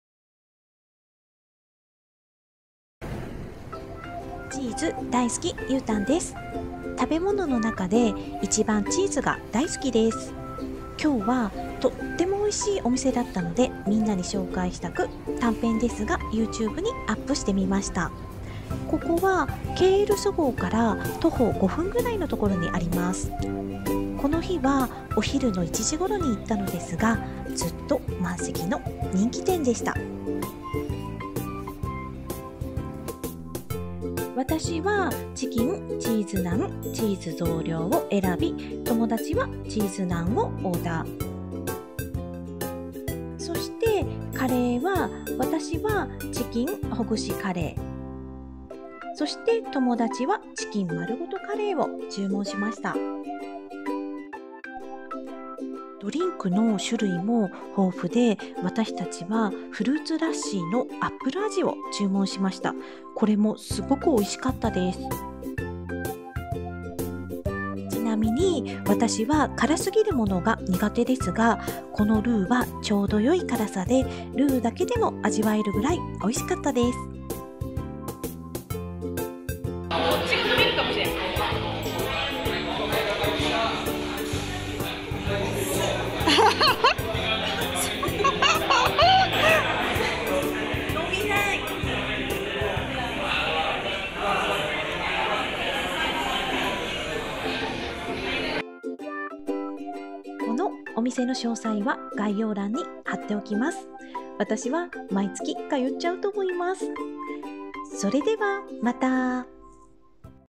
【マレーシア🇲🇾】💕I Love Cheese💕チーズ好きな方にオススメしたいお店（チーズナン）です😊（超短編だよ） #13
チーズナン好きな人必見💕美味しすぎて皆さんに伝えたくお店のご紹介です↓
【RSMY - House of Real Beriani】
※You Tube用に撮影する予定はなかったので、毎回やってる「はろはろ〜！ゆーたんです！」を撮ってない（笑）＆写真ばかりの短編です😅

近々マレーシア渡航予定の方、マレーシアに興味がある方、私の生存状況が気になる方😉是非ご覧下さい。
【I LOVE CHEESE💕】
Introducing my favorite 「Cheesenaan」 shop😘